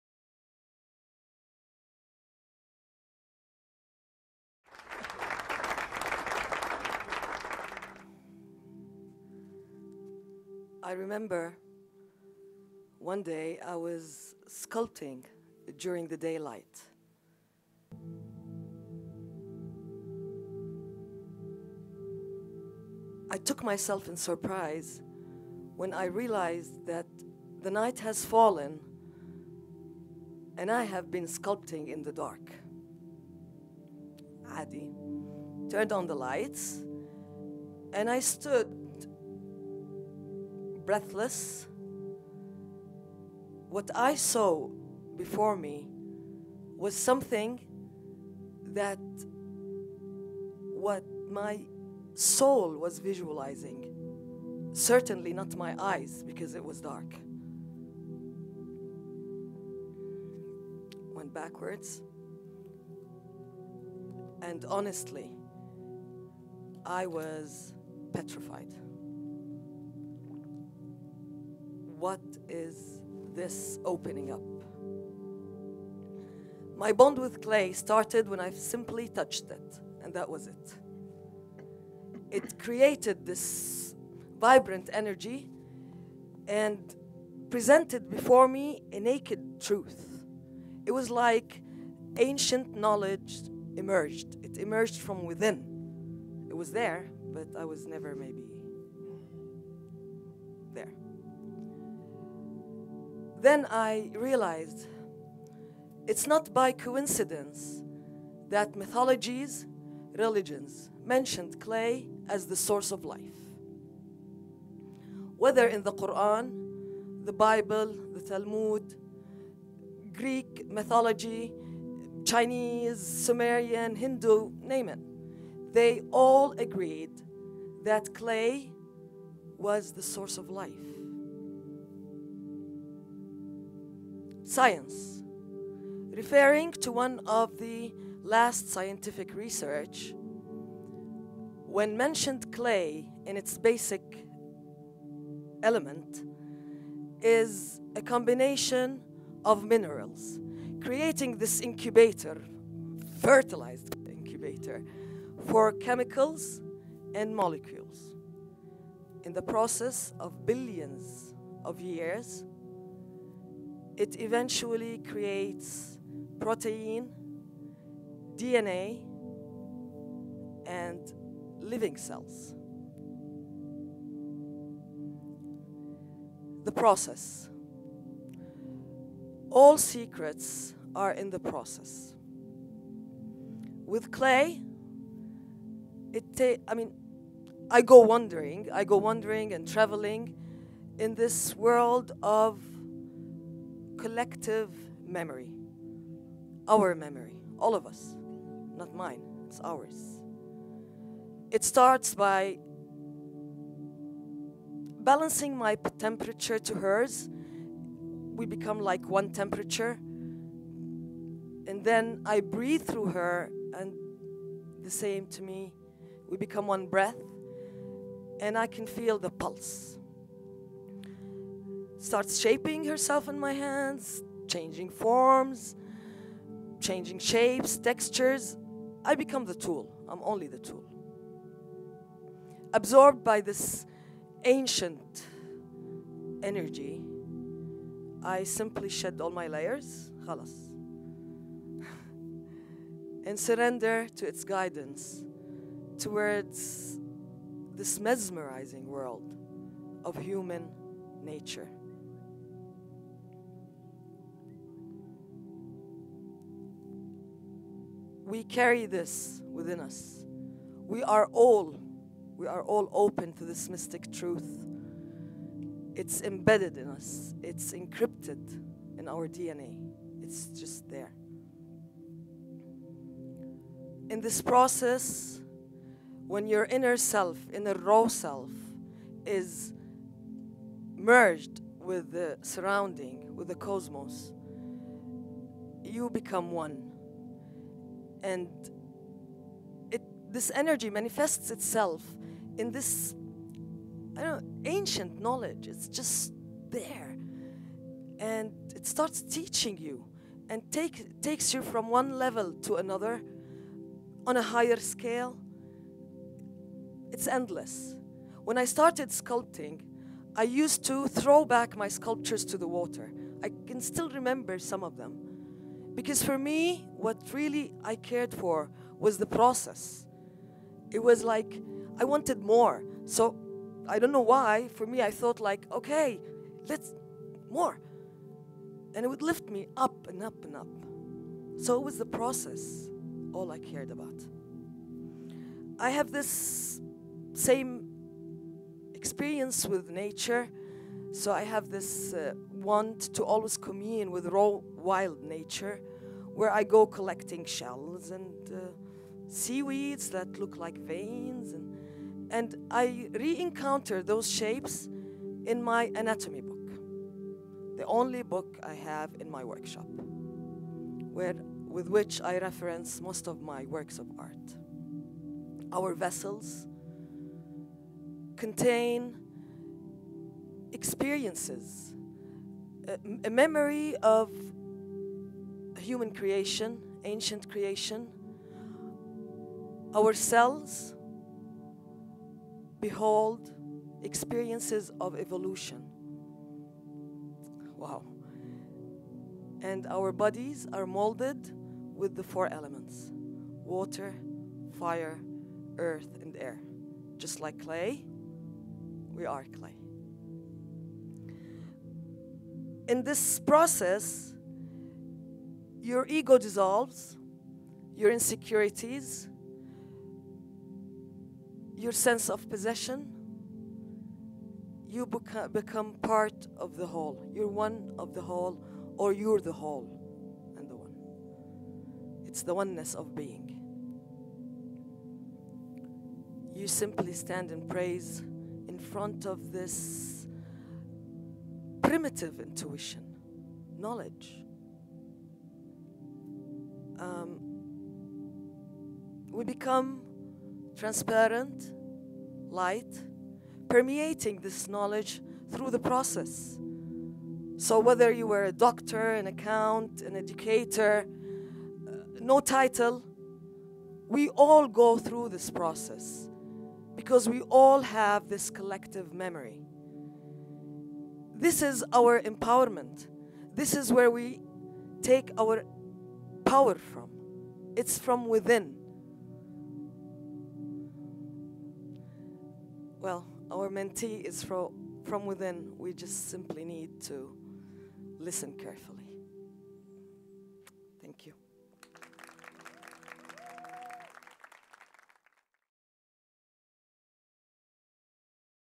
Pulse | Katia Al Tal | TEDxArarStreetWomen
Katia Al Tal’s speech constitutes the pillars of an
eternal bond through the idea of vibrating energy.
The naked truth it takes when we pour our souls
into the process. Once absorbed authentic
knowledge can present itself before us.
All secrets are in the process. Clay allows us to
travel in the collective memory, starting with
balance, pulse and breath, it becomes a living
creature and gives its guidance towards the
mesmerizing truth of human nature. We are all
open to this mystic truth, for we all carry this
primitive instinct. It’s within every one of us, it’s
encrypted in our own DNA.  This talk was given at a TEDx event using the TED conference format but independently organized by a local community.